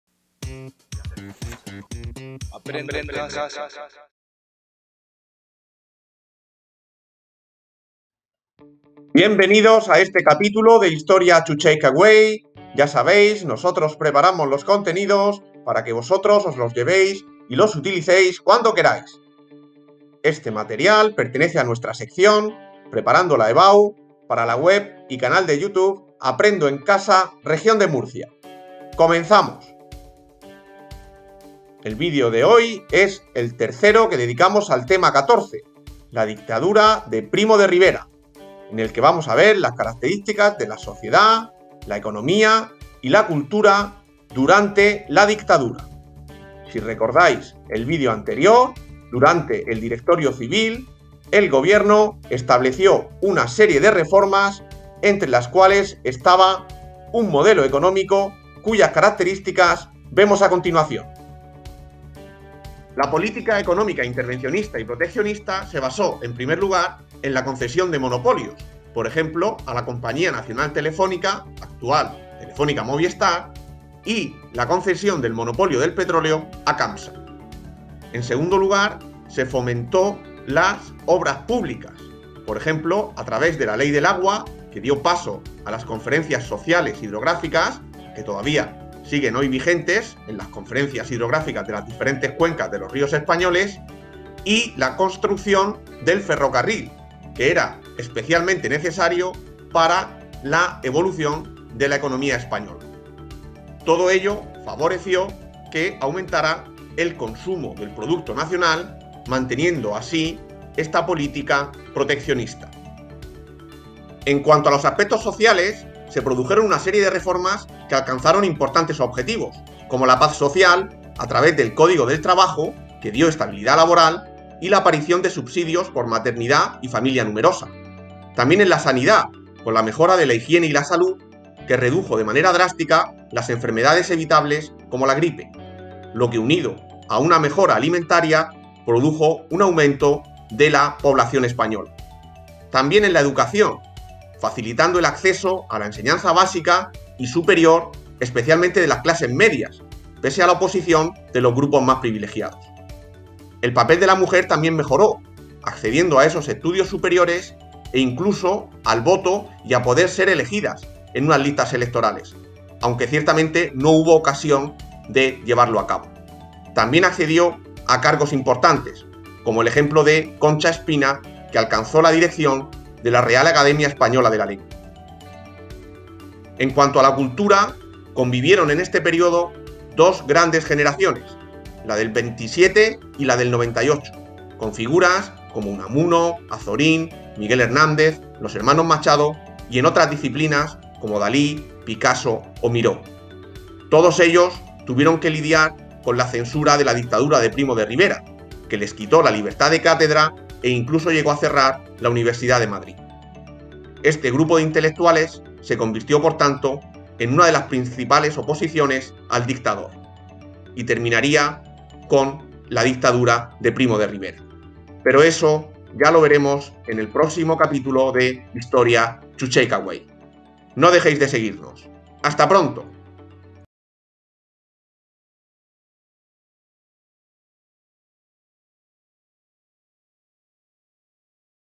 LA DICTADURA DE PRIMO DE RIVERA (3ª parte). ASPECTOS SOCIALES, ECONÓMICOS Y CULTURALES
Video explicativo sobre los aspectos sociales, económicos y culturales durante la dictadura de Primo de Rivera.